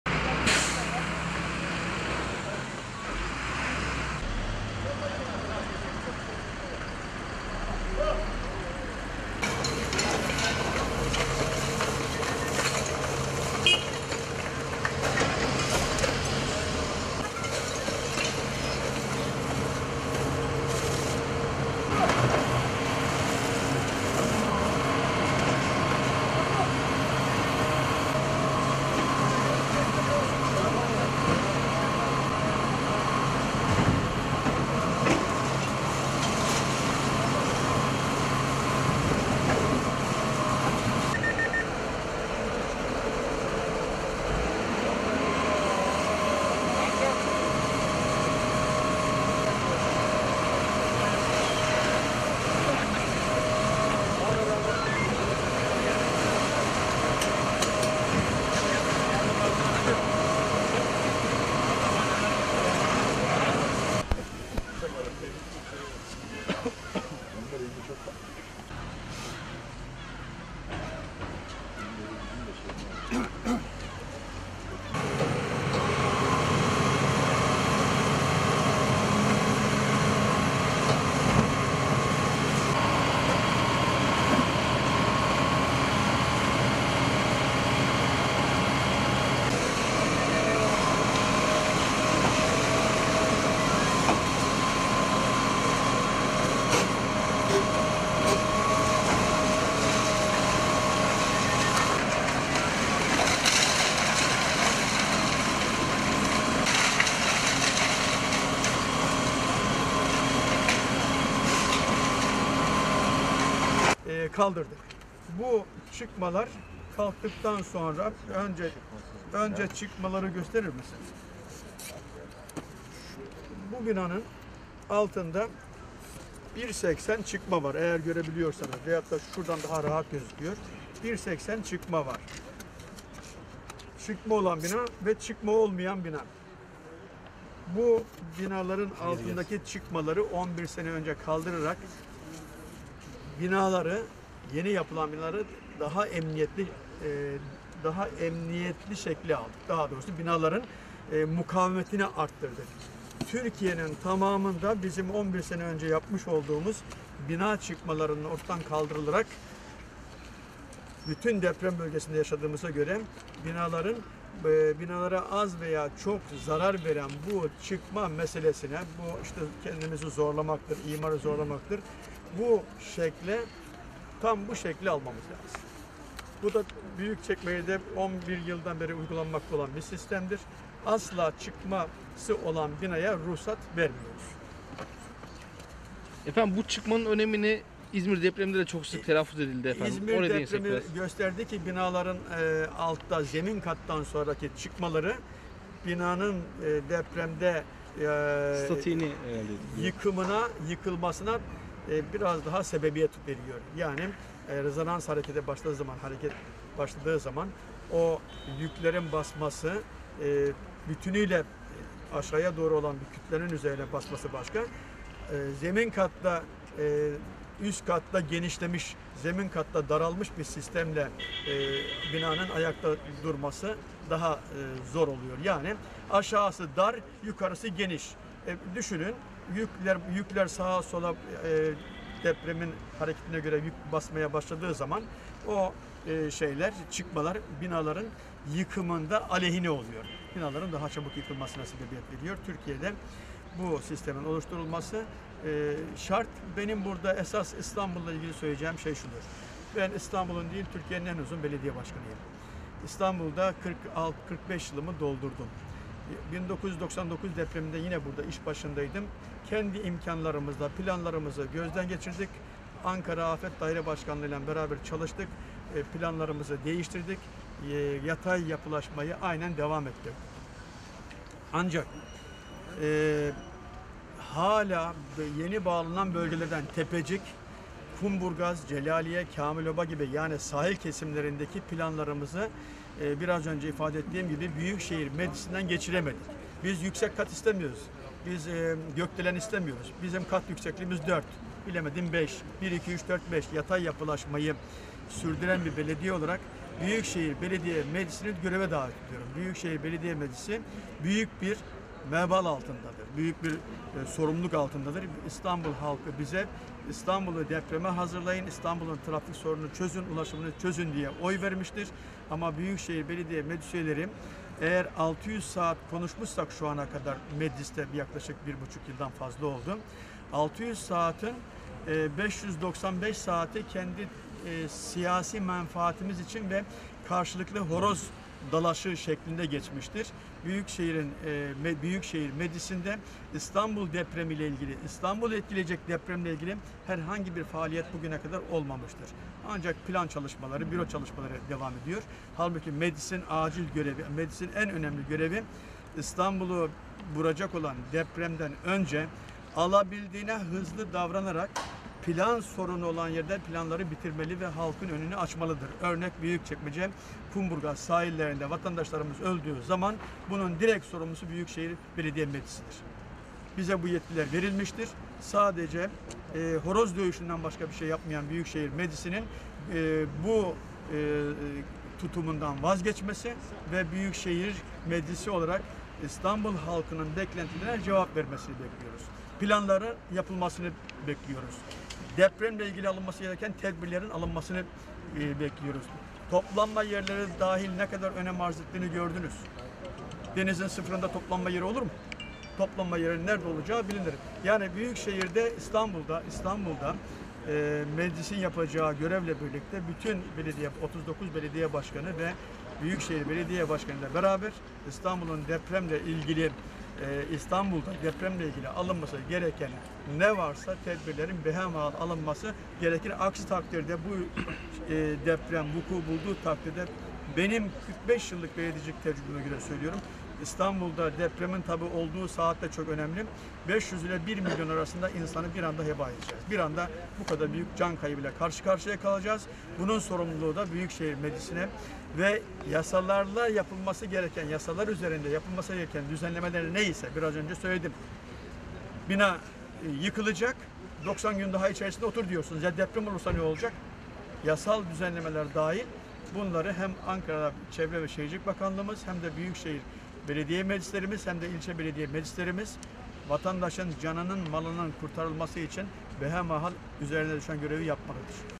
İzmir Depreminin Ardından Büyükçekmece’de Hasarlı Binaların Yıkımına Devam Edildi
Büyükçekmece’de geçtiğimiz yıl meydana gelen 5.8’lik Silivri depreminin ardından hasarlı binanın yıkımı yapıldı. Binanın yıkımını yerinde inceleyen Büyükçekmece Belediye Başkanı Dr. Hasan Akgün, İstanbul Büyükşehir Belediyesi Meclisinin sorumluluklarını depremle ilgili çalışmalarını eleştirerek, “İBB Meclisi büyük bir vebal ve sorumluluk altındadır” dedi.
İzmir’de meydana gelen 6.6 şiddetindeki depremin ardından Büyükçekmece’de devam eden kentsel dönüşüm çalışmaları bir kez daha dikkat çekti. 19 Mayıs Mahallesi’nde bulunan 7 ayrı binada yıkım çalışmaları başladı. Yıkımı yapılan binalardan birisinin geçtiğimiz yıl Eylül ayında Silivri açıklarında meydana gelen 5.8 şiddetindeki depremde hasar gören ve boşaltılan 34 yıllık bina oldu. Büyükçekmece Belediye Başkanı Dr. Hasan Akgün de yıkım çalışmalarını yerinde inceledi. Başkan Akgün, yetkililerden bilgiler aldı. Akgün, binaların yapımı sırasında dışarıya sarkan çıkmaların da büyük risk oluşturduğunu söyleyerek, “Binanın altında bir 80’lik bir çıkma var. Bu binaların altındaki çıkmaları 11 sene önce kaldırarak binaların mukavemetini artırdık. Türkiye’nin tamamında bizim 11 sene önce başlattığımız bina çıkmalarını ortadan kaldırarak binalara az veya çok zarar veren bu çıkma meselesinin halletmemiz lazım. Çıkması olan binalara ruhsat vermiyoruz” dedi.
İzmir’deki depremde hasar alan binaların birçoğunda çıkma tabanlar olduğunu hatırlan Akgün, “Binanın yıkılmasına sebebiyet veriyor bu çıkmalar. Binaların ayakta durması daha zor oluyor. Aşağısı dar yukarısı geniş oluyor. Yükler deprem hareketi ile sağa sola basmaya başladığında bu çıkmalar binaların aleyhine oluyor. Türkiye’de bu sistemin oluşturulması şart” diye konuştu.
“İBB Meclisi büyük bir vebal ve sorumluluk altındadır”
Akgün, sahil kesiminde bulunan mahallerinin planlarını İstanbul Büyükşehir Belediyesi (İBB) Meclisinden geçiremediklerini ifade ederek, “ Biz yüksek kat ya da gökdelen istemiyoruz. Yatay yapılaşmayı sürdüren bir belediye olarak İBB meclisini göreve davet ediyorum. İBB Meclisi büyük bir vebal ve sorumluluk altındadır. Bize İstanbullu, İstanbul’u depreme hazırlayın trafiğe hazırlayın diye oy vermiştir ama İBB meclis üyeleri 600 saat konuştuysa bunun 595 saati kendi siyasi menfaati ve karşılıklı horoz dalaşı şeklinde geçmiştir. Mecliste İstanbul depremi ile ilgili herhangi bir faaliyet bugüne kadar olmamıştır. Ancak plan ve büro çalışmaları devam ediyor; ancak meclisin en önemli görevi alabildiğine hızlı davranarak plan sorunu olan yerlerde planları bitirmeli ve halkın önünü açmalıdır. Örneğin Büyükçekmece Kumburgaz sahilinde vatandaşlarımız öldüğü zaman bunun direkt sorumlusu İBB Meclisidir. Horoz dövüşünden başka bir şey yapmayan İBB Meclisinin bu tutumundan vazgeçmesi ve İstanbul halkının beklentilerine cevap vermesi gerekmektedir” şeklinde konuştu.